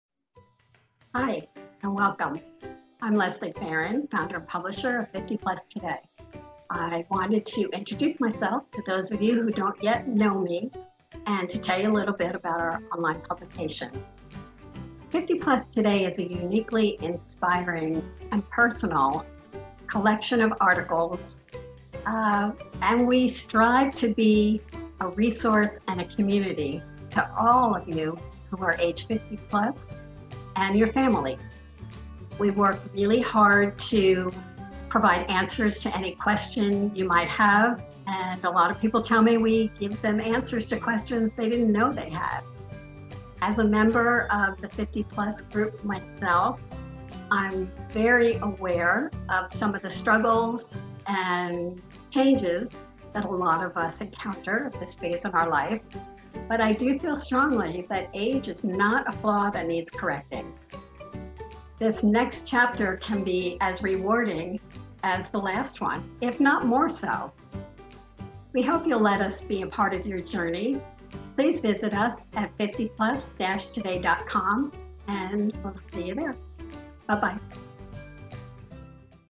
50plustoday: WELCOME
About 50Plus-Today.com: Leslie Farin, Founder and Publisher,  provides an overview of her online magazine to welcome people to the 50PlusToday community.